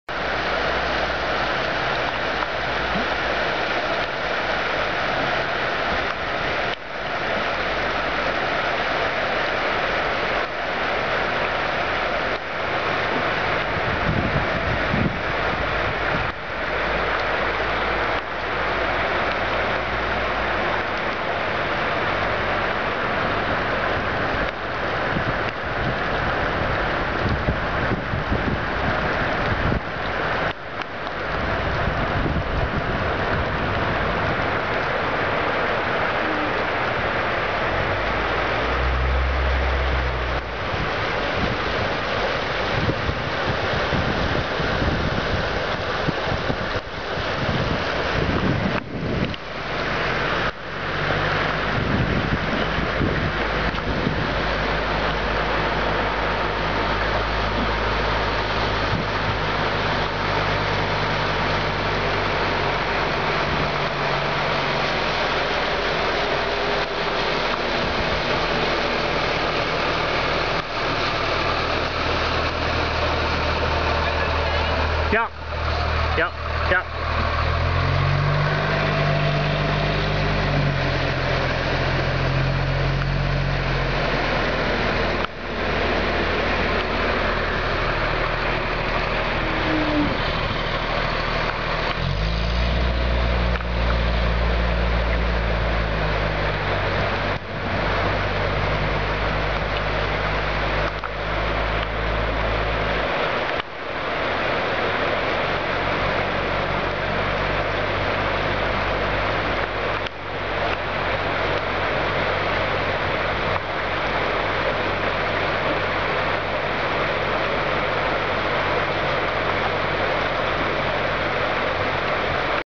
Truro Flood - Sept. 10th - Park St.
Park St, CEC, Colchester Stadium and area flood after storm and levee break.